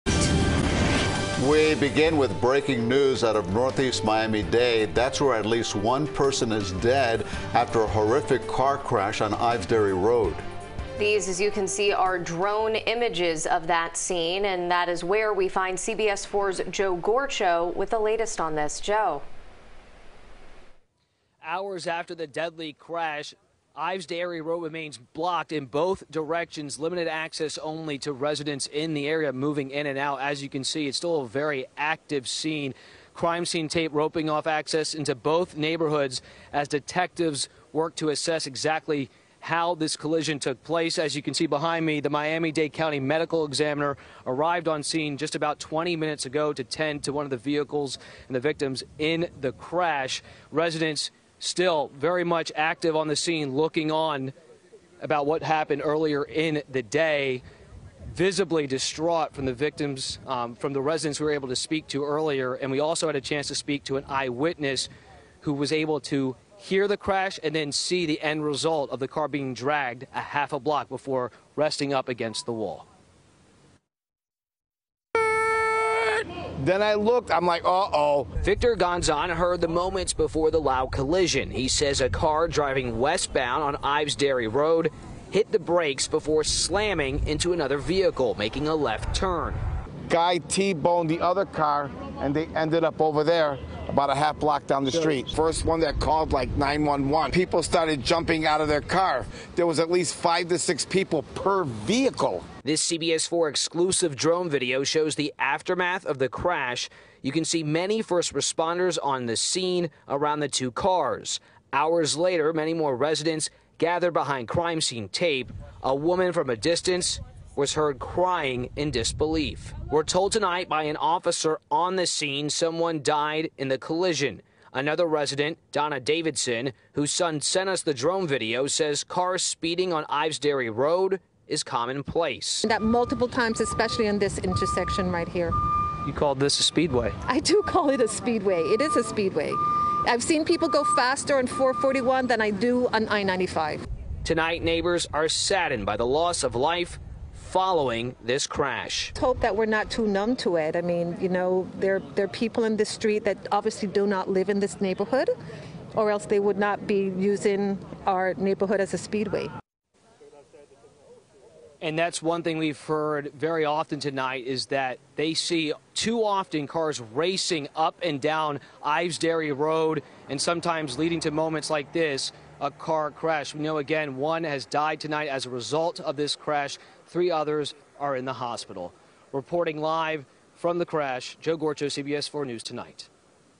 Deadly Car Crash On Ives Dairy Road Under Investigation, Neighbors Complain Roadway Used To Race
CBS4's Joe Gorchow reports on a fatal crash that occured in Northeast Miami on Friday night.